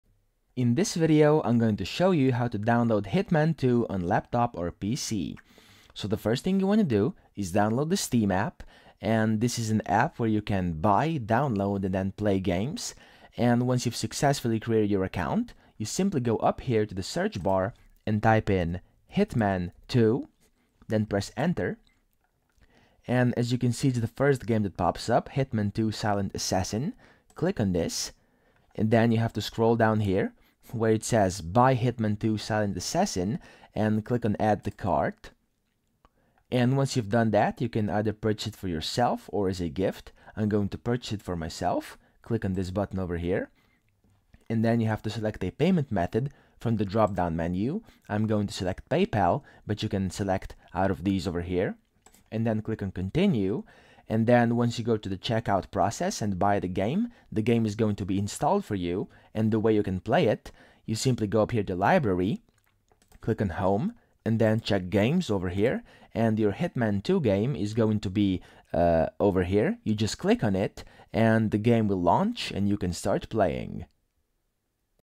How To DOWNLOAD Hitman 2 On PC/Laptop (2026)
In this video, I will show you how to download Hitman 2 on PC/Laptop in 2026. This is a quick and easy tutorial on downloading Hitman 2 to your computer from Steam.

In case you've been wondering how to download and install Hitman 2, this is the video for you!